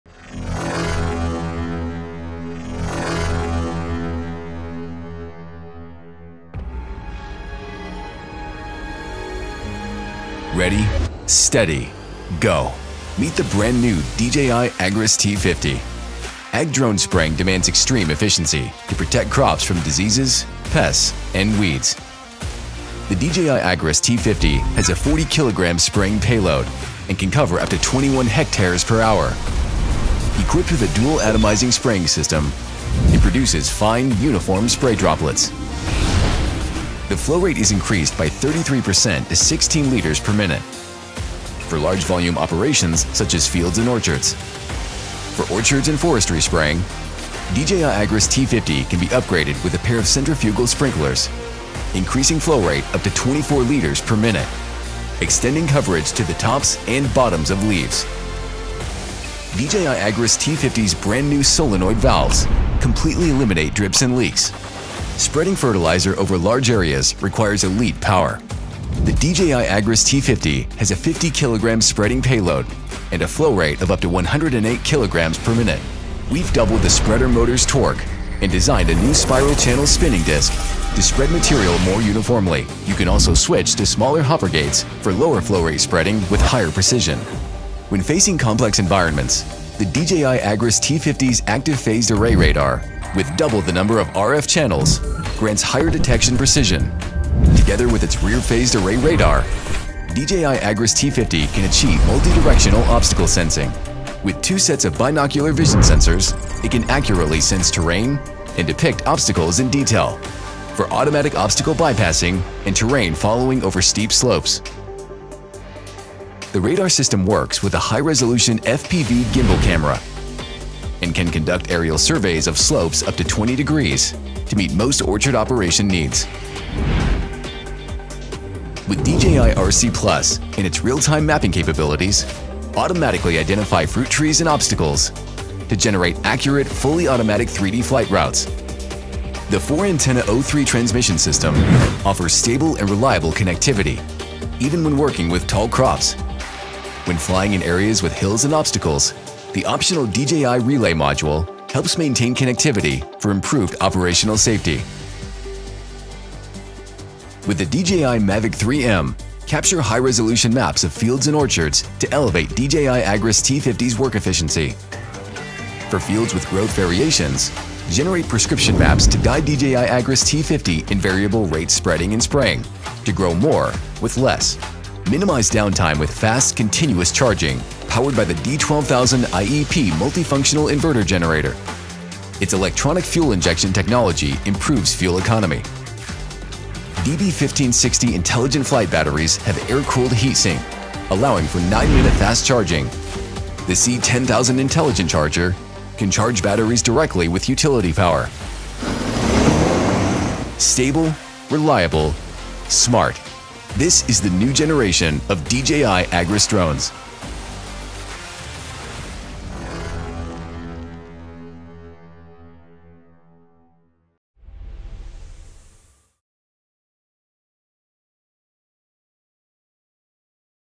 Meet the DJI Agras T50 Spray Drone
The Agras T50 elevates drone agricultural operations to new heights. It inherits a powerful coaxial twin-rotor propulsion system and a split-type torque resistant structure for next level stability when carrying 40 kg/88 Lb. spraying or 50 kg/110 Lb. spreading payloads. T50 leverages a Dual Atomizing Spraying System, Front and Rear Phased Array Radars, and a Binocular Vision System. T50 excels across multiple scenarios, from surveying to spraying and spreading, guaranteeing stable operations and steady performance.

Heavy Payload
• 40 kg/88 Lb. spraying
• 50 kg/110 Lb. spreading

High Flow Rate
• Spraying 16 L (4.23 Gal)/min
• Spreading 108 kg (238 Lb)/min

Signal Stability
• Offline Operations
• 2 km O3 Video Transmission
• Optional DJI Relay

All Scenario Adaptability
• Automatic and Manual Orchard Mode
• Variable Rate Application

Multidirectional Obstacle Sensing
• Multidirectional Obstacle Avoidance
• Terrain Following up to 50°

Four Sprinkler Kit (Optional)
• Reverse Directional Spray
• 4-Sprinkler Spraying 24 L (6.34 Gal)/min Flow Rate

High Rate, Atomized Spray, Leak Free
• Magnetic Drive Impeller Pump,
• Dual Pump Flow Rate of up to 24 L/min[3]
• Dual Atomizing Centrifugal Sprinklers, Adjustable Droplet Size
• Brand-New Solenoid Valves, Leak Free
• Two centrifugal sprinklers can be incorporated to ensure an effective spray coverage on fruit trees. This process can be performed manually, eliminating the need for rotation during operation

1,500 kg Spreading Per Hour
• Efficiency spreading, flow rate up to 108 kg/min[4]
• Smooth spreading, doubled torque of the spreader
• Uniform spreading, spiral channel spinning disk
• Low rate spreading, small hopper gates
• Real-Time Weighing
• Fast Disassembly and Cleaning

Double Peace of Mind with Binocular Vision and Dual Radar
• Front and Rear Active Phased Array Radars
• Two Sets of Binocular Vision Sensors
• Multidirectional Obstacle Sensing and Avoidance
• Terrain Following up to 50°, Automatic Obstacle Bypassing

One Drone for Surveying, Spraying, and Spreading
• High Resolution FPV Gimbal Camera
• The angle of the gimbal is adjustable and images can be collected in real time
• Orchard Aerial Survey, 5 hectare orchard mapped in 10 minutes
• Field Aerial Survey, 13 hectare field mapped in 10 minutes
• Surveying of slopes up to 20°

DJI RC Plus
• Quad-antenna O3 Transmission, up to 2 km range
• 7-inch high brightness screen
• 8 core processor for smooth operations
• Optional DJI Relay Module

Fuel-Efficient Fast Charging
• 9-min ultra-fast charging
• 1,500 W AC output
• Warranty covers 1,500 charging cycles
• 1.5 m extra-long charging cable